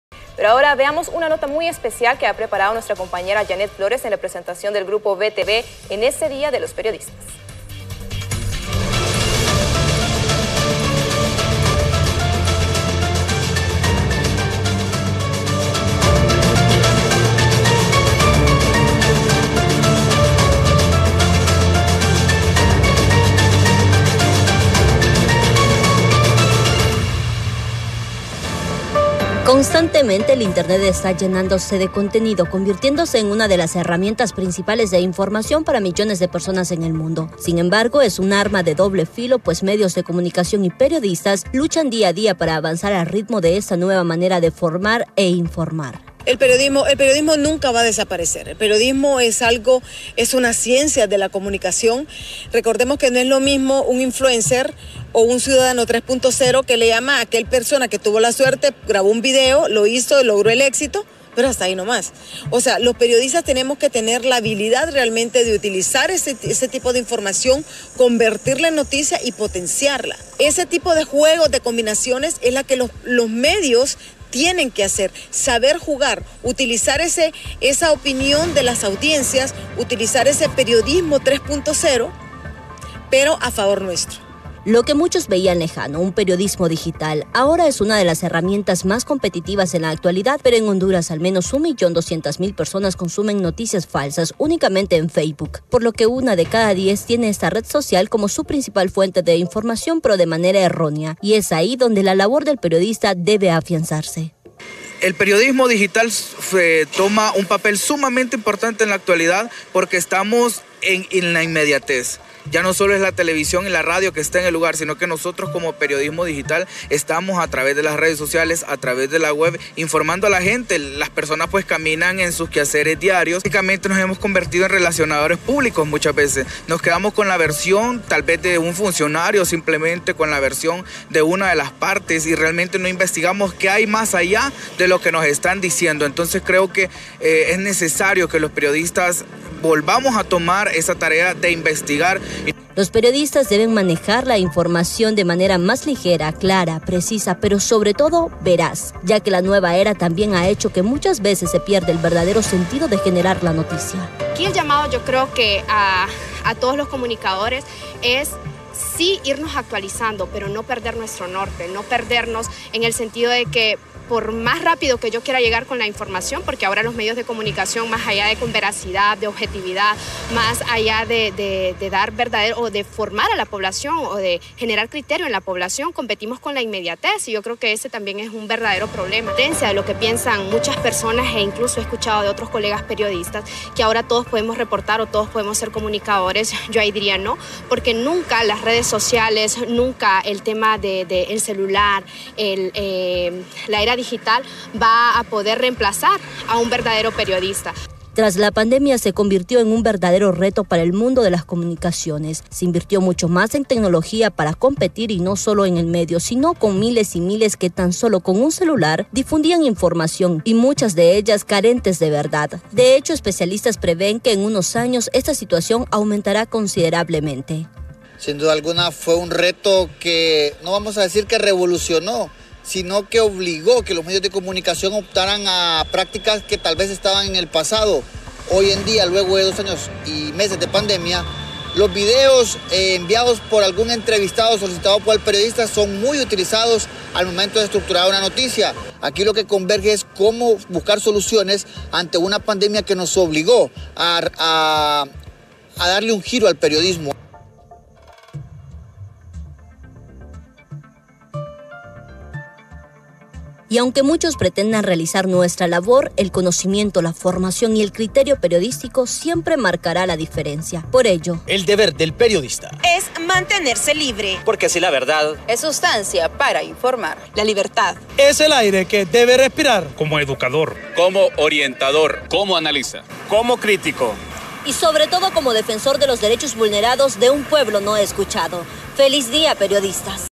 Honduras conmemora el día del periodista
No es fácil la labor, pero el periodista hondureño dignifica a diario su profesión
Varios sectores del país se unen a las felicitaciones, ya que hoy en Honduras, se reconoce la labor periodística. --